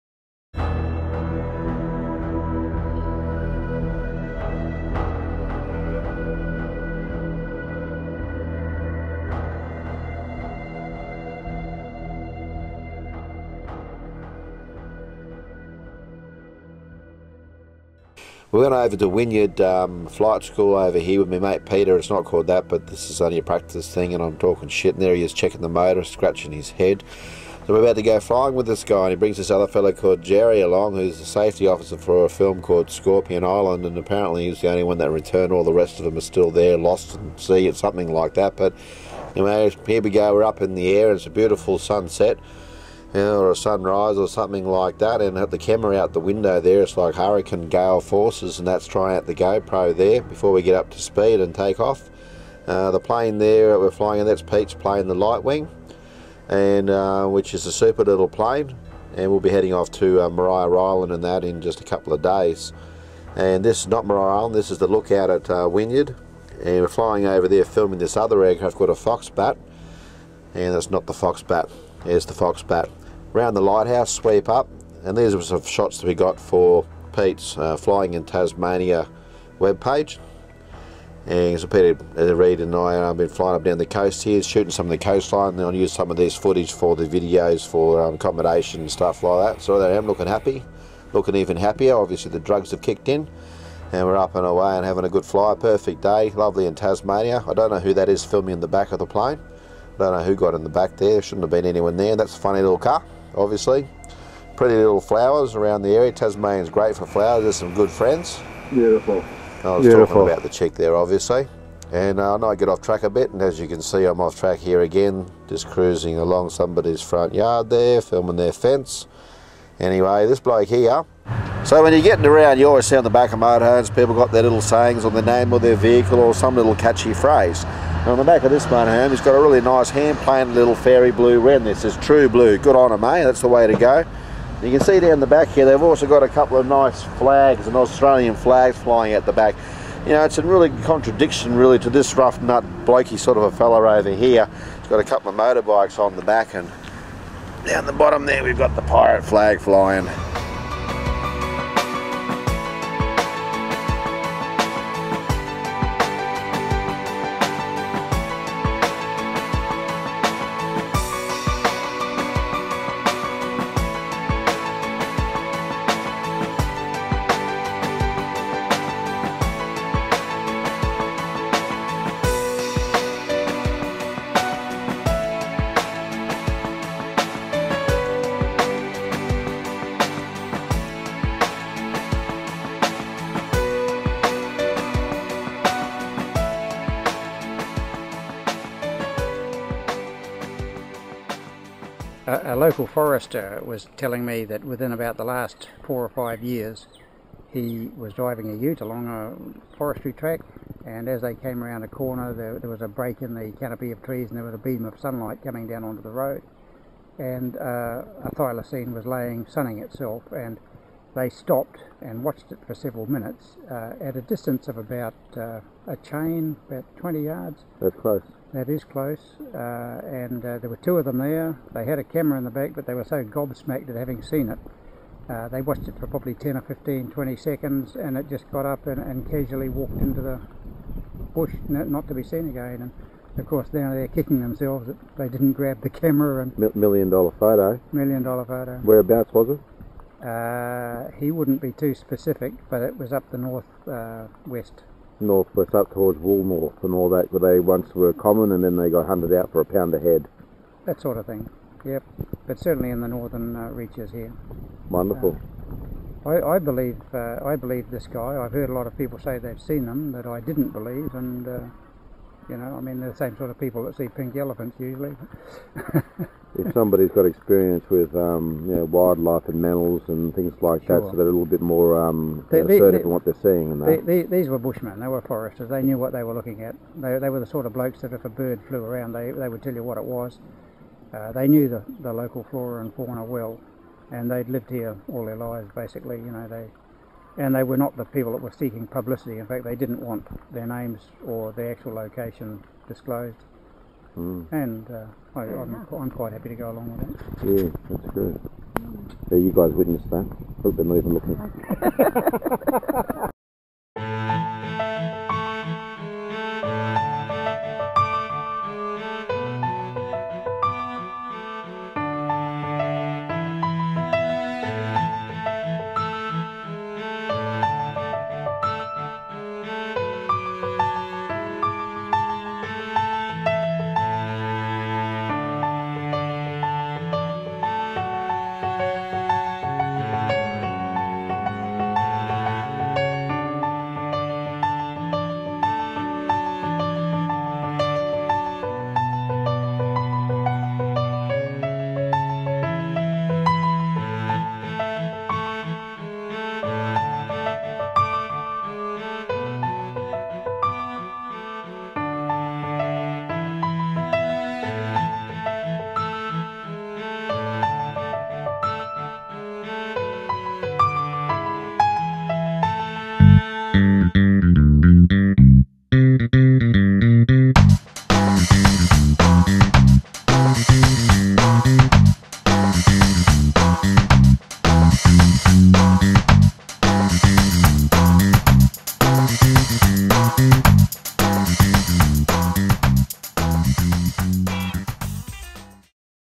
TRAVEL AUSTRALIA 10 THYLACINE STORY AND FLYING IN TASMANIA WYNYARD
In this video i go flying in tasmania along the north coast with Peter and film an aeroplane called a Foxbat. There is also a recording i did of a live account of the extinct Thylacine in the north west coast near Woolnorth, Tasmania.